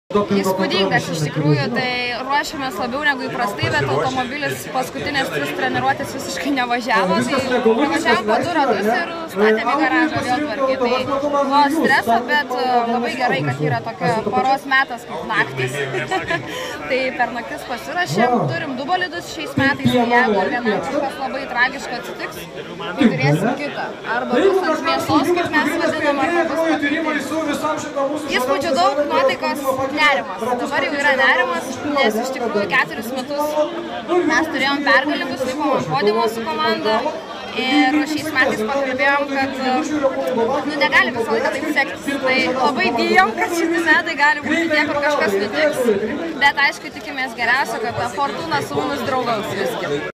Lenktynininkė Aistė Matukaitė: „Arba bus ant mėsos, arba labai gerai“
Ketvirtadienį, liepos 20-ąją, Palangos Vytauto gatvėje spalvingas lenktyninių automobilių paradas susirinko į aštuonioliktųjų „Eneos 1006 km lenktynių“ atidarymą. Čia dalyvių laukė ir pirmosios varžybos - labai trumpos dinamiškos „Dynami:t Super sprint“ lenktynės. Iki jų starto likus dar kelioms valandoms maratoninių lenktynių gerbėjai margaspalviame sportininkų parade ieškojo ne tik pažįstamų veidų, bet ir apžiūrinėjo lenktynininkų techniką bei mėgino prognozuoti šeštadienį vyksiančių 1006 km. lenktynių baigtį.